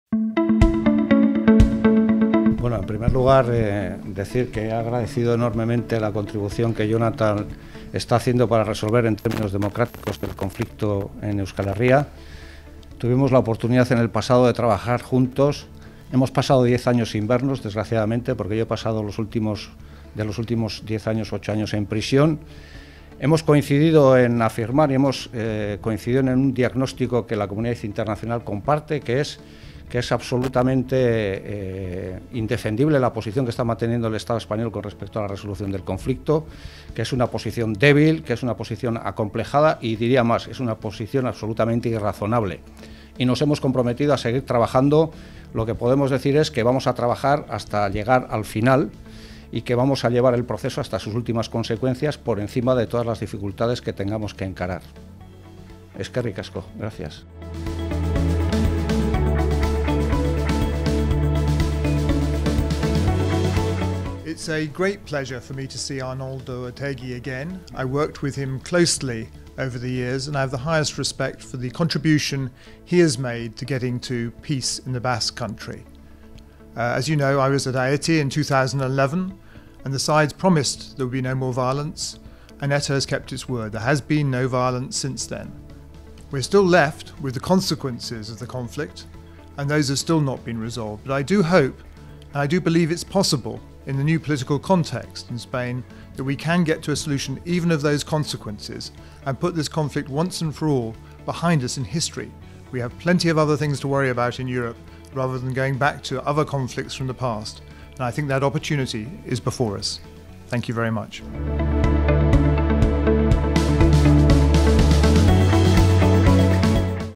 OtegiEuropeanTour Declaraciones de Arnaldo Otegi y Jonathan Powell tras la reunión
Después de 10 años, Arnaldo Otegi y Jonathan Powell han mantenido un encuentro, esta mañana, en Londres. En declaraciones realizadas ante los medios de comunicación tras la reunión, ambos han mostrado su compromiso de seguir trabajando juntos para buscar la solución de las consecuencias del conflicto.